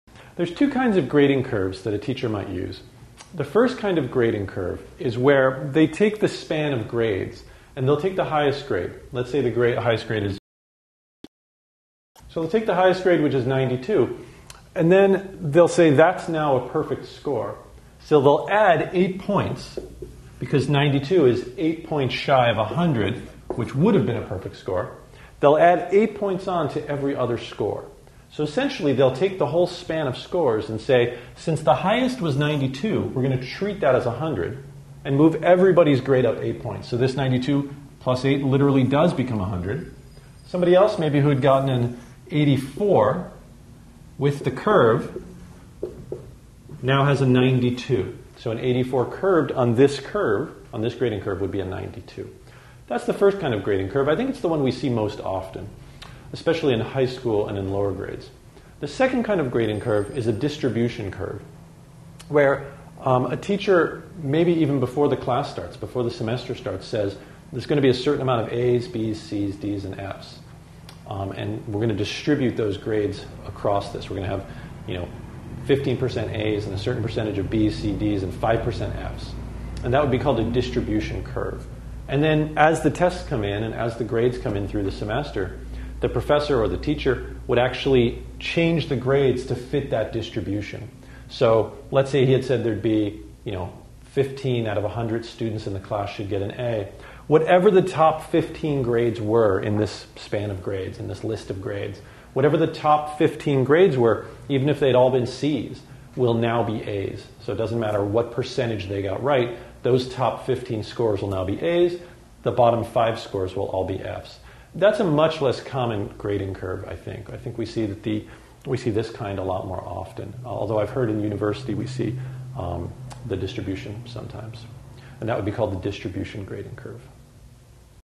How Does a Grading Curve Work?
How Does a Grading Curve Work?. Part of the series: Math Lessons & Study Tips. A grading curve usually works in one of two ways, which are called bell curves or distribution curves. Find out what grading curves are generally used in college with help from a tutor in this free video on math lessons and study tips.